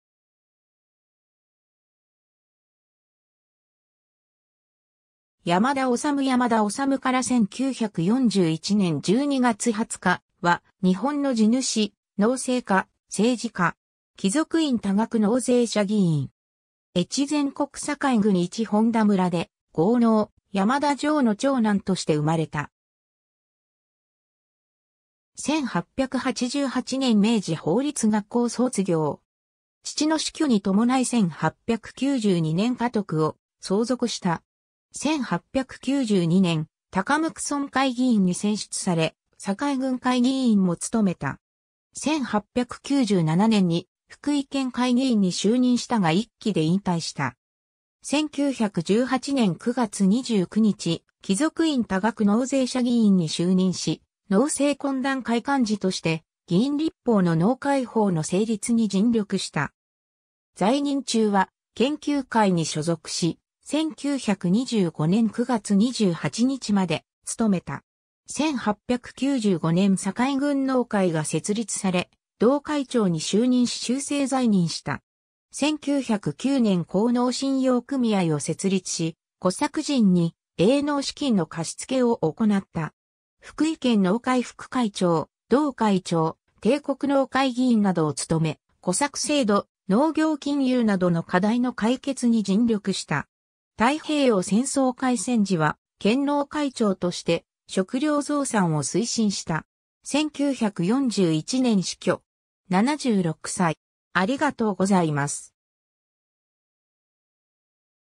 山田斂
山田斂 山田 斂（やまだ おさむ、1865年10月8日（慶応元年8月19日）- 1941年（昭和16年）12月20日）は、日本の地主、農政家、政治家。
貴族院多額納税者議員。
 越前国坂井郡一本田村（福井県坂井郡丸岡町を経て現坂井市）で、豪農・山田穣の長男として生まれた。
1888年（明治21年）明治法律学校（現明治大学）卒業。
父の死去に伴い1892年（明治25年）家督を相続した。
 1892年、高椋村会議員に選出され、坂井郡会議員も務めた。
1897年（明治30年）に福井県会議員に就任したが1期で引退した。
1918年（大正7年）9月29日、貴族院多額納税者議員に就任し、農政懇談会幹事として議員立法の農会法（大正11年4月12日法律第40号）の成立に尽力した。
在任中は研究会に所属し、1925年（大正14年）9月28日まで務めた。
 1895年（明治28年）坂井郡農会が設立され、同会長に就任し終生在任した。
1909年（明治42年）興農信用組合を設立し、小作人に営農資金の貸付を行った。
福井県農会副会長、同会長、帝国農会議員などを務め、小作制度、農業金融などの課題の解決に尽力した。
 太平洋戦争開戦時は、県農会長として食糧増産を推進した。
1941年死去。
76歳。